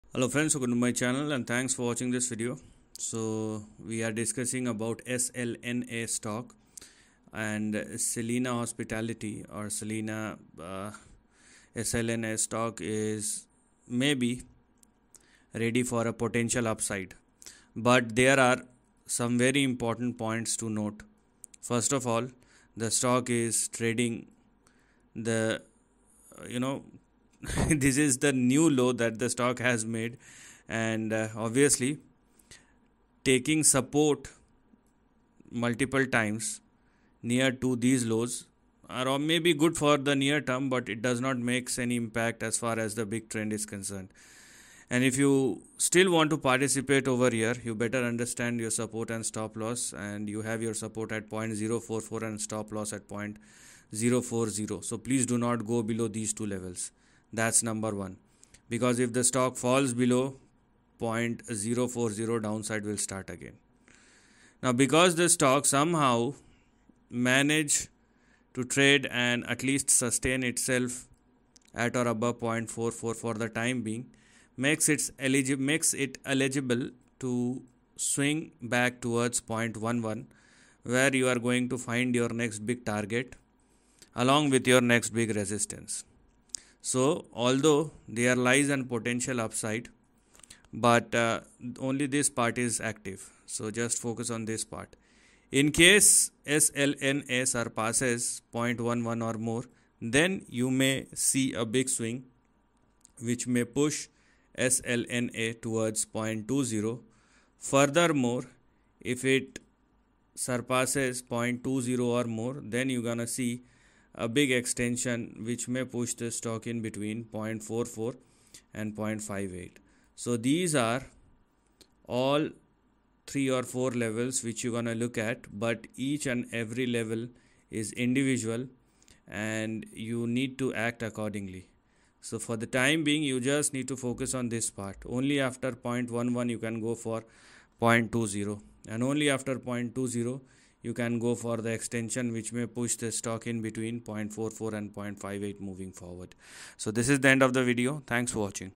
POTENTIAL UPSIDE | $SLNA STOCK ANALYSIS | SELINA HOSPITALITY STOCK
In this video, we will take a closer look at SLNA STOCK ANALYSIS which is in a POTENTIAL UPSIDE for the next couple of days

BEST BOOKS LINKS TO BECOME PRO INVESTOR/TRADER :
FOR EQUITIES: